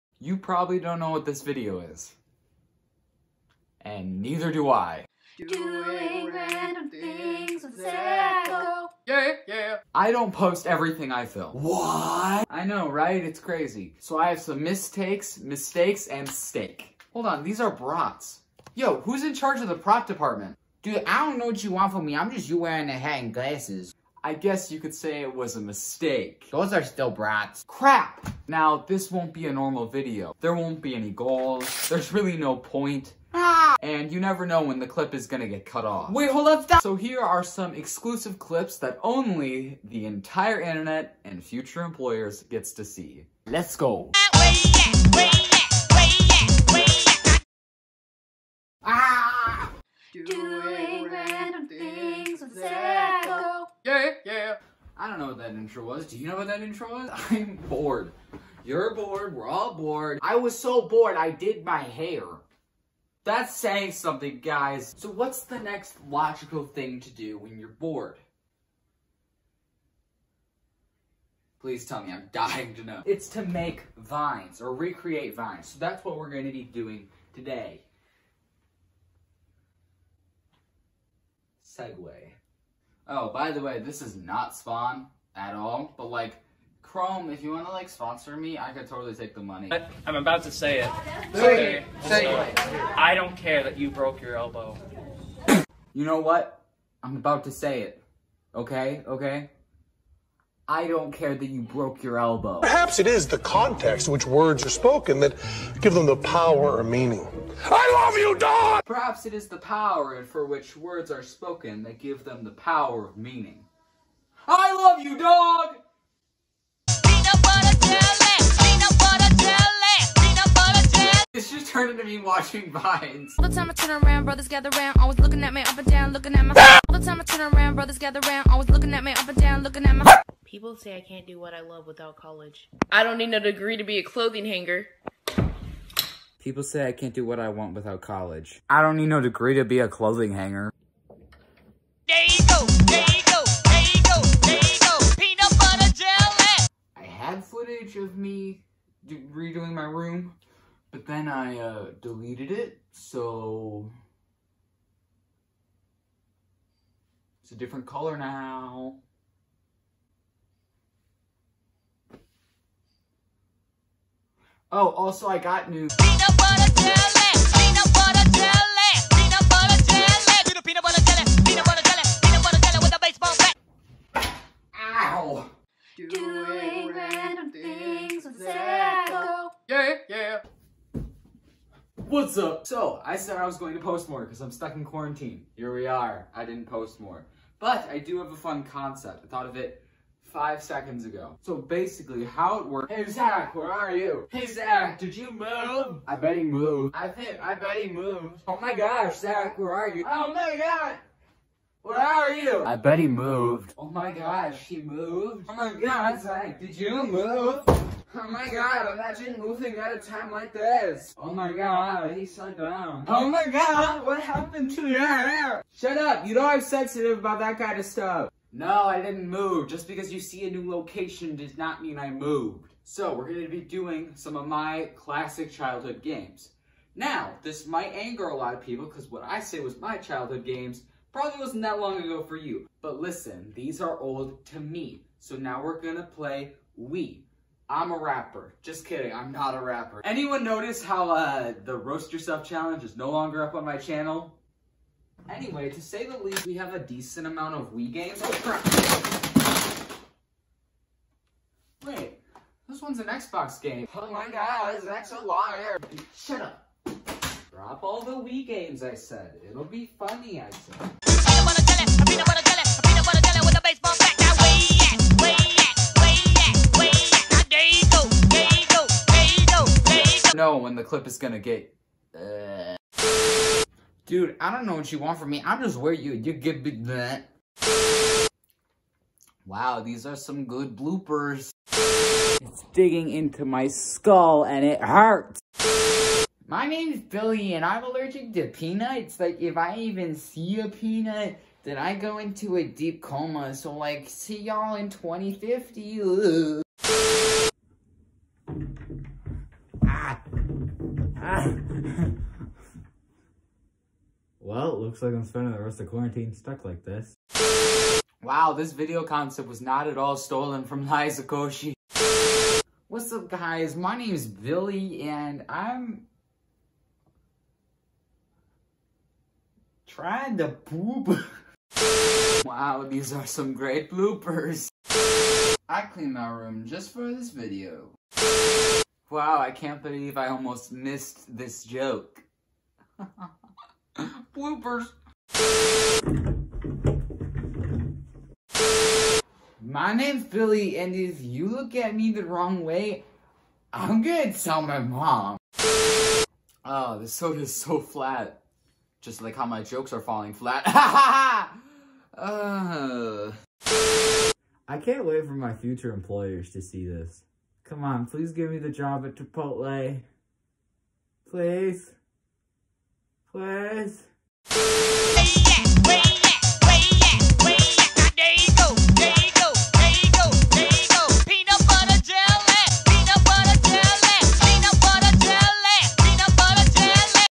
The Videos I Never Posted...
Some videos that weren't funny, like at all. But I'm out of ideas so tried to pull together whatever clips I could. Enjoy?